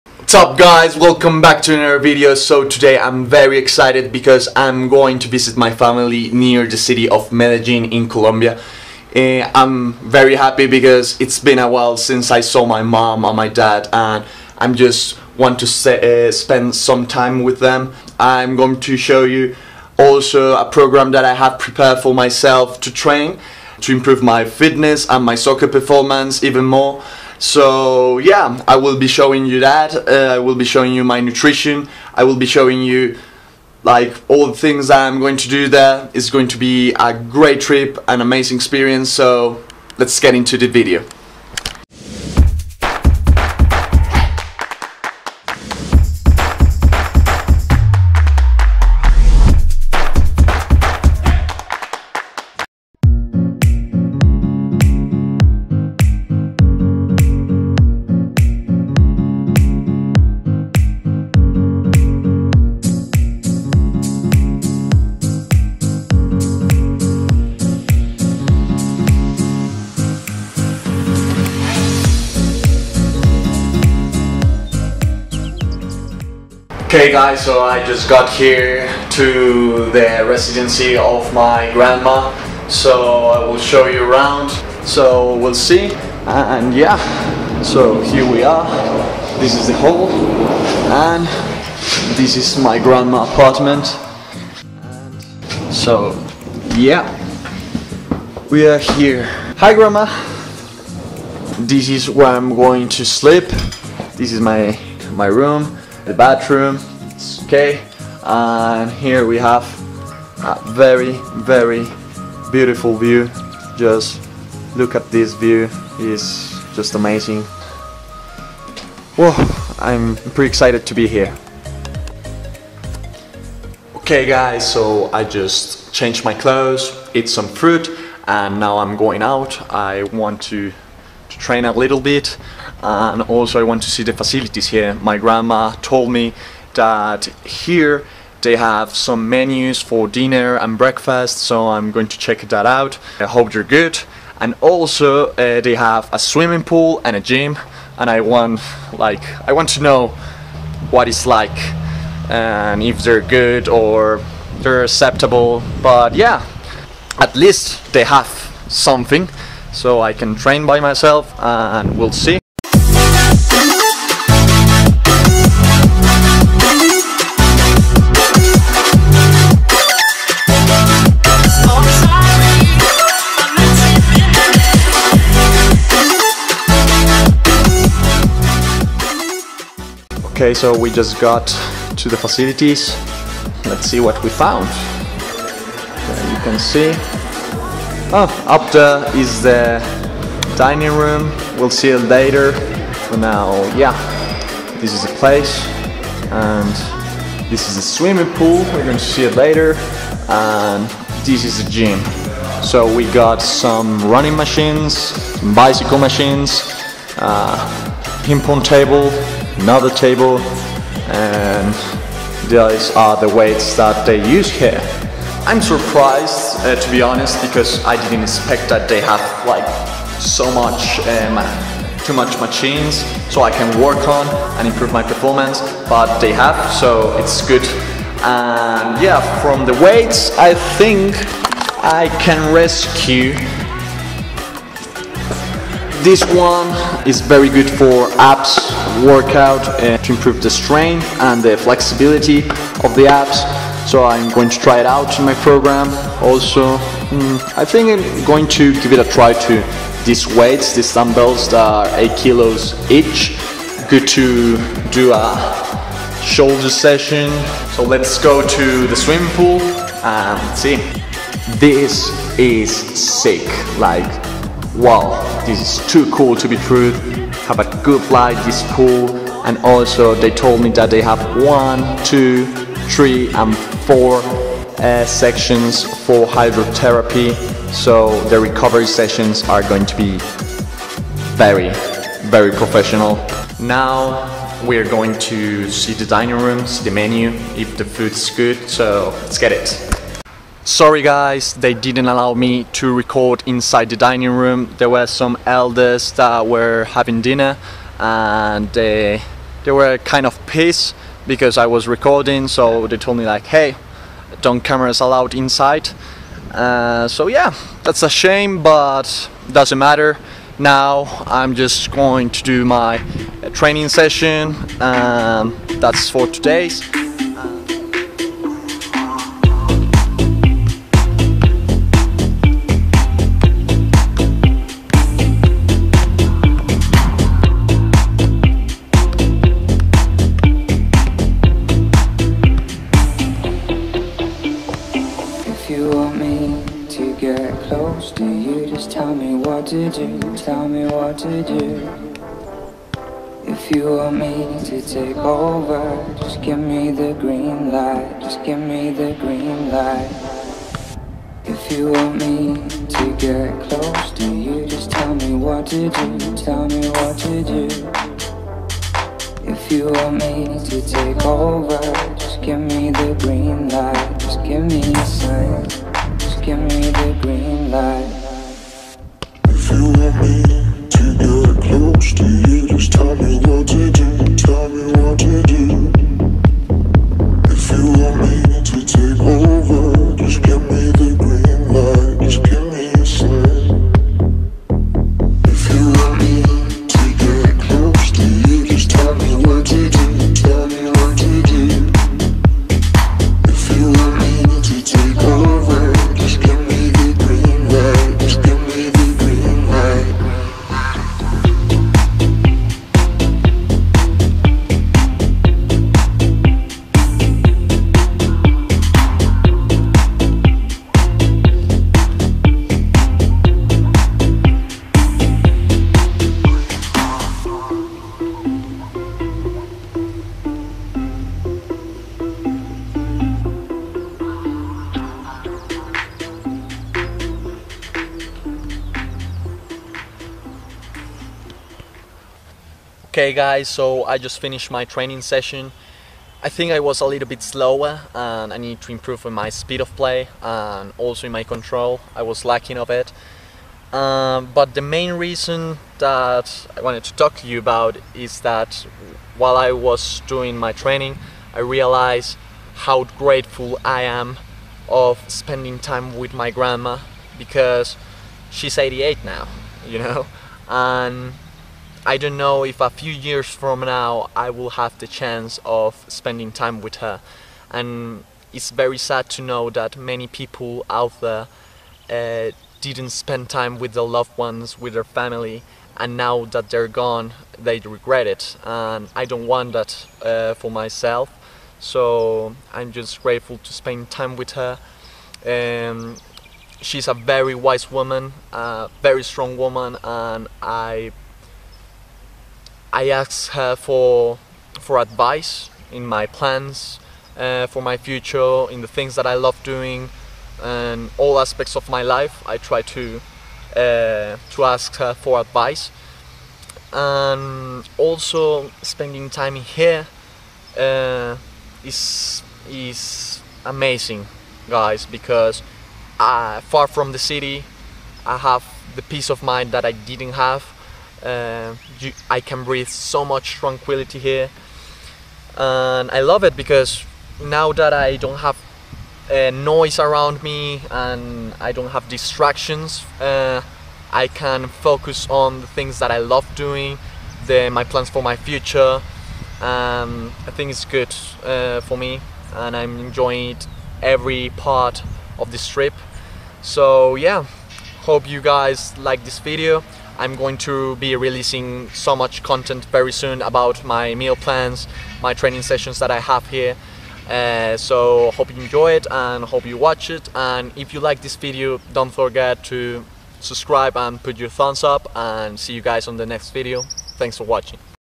New Beginning, New Adventures! Working For my Trials
In this video I will show you my thoughts about this new journey that I'm living, Showing you different things about the place where I'm going to stay, with a message at the end for all of those that still have there love ones alive.